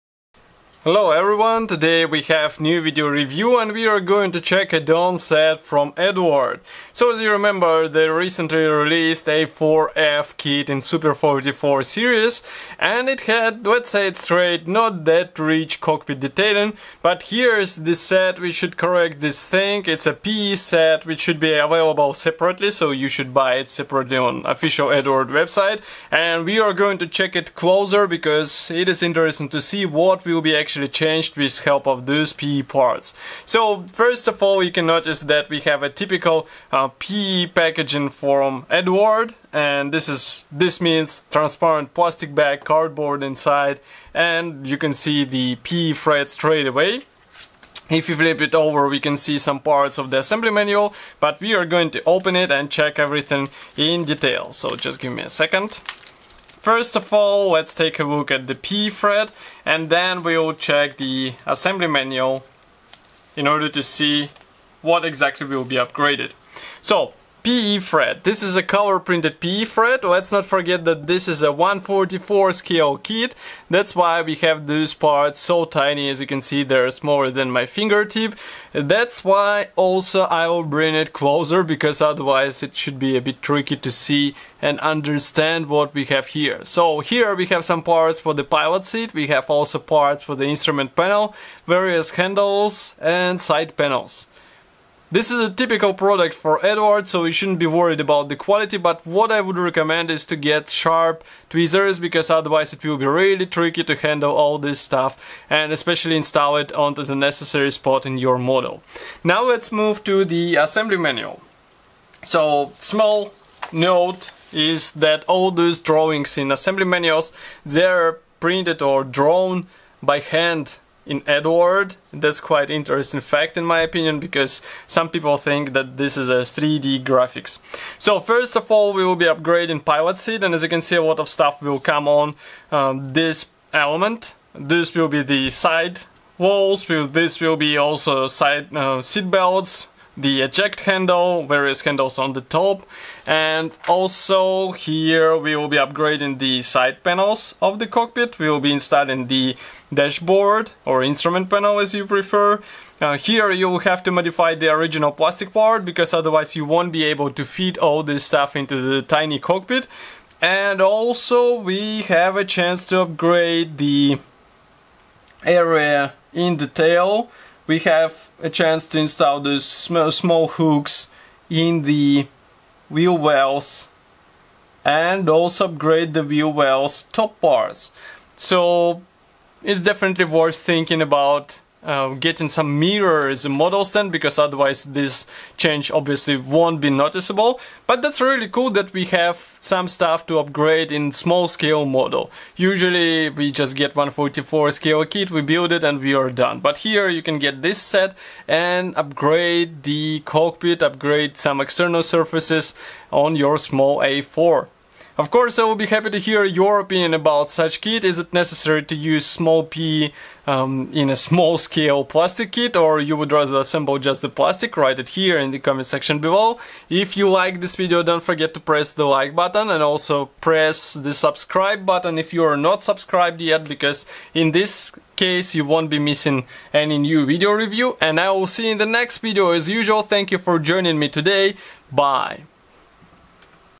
Eduard 1/144 A-4F PE (144010) Review
Today we are going to check new PE set from Eduard. This upgrade should come handy for their 1/144 A-4F kit.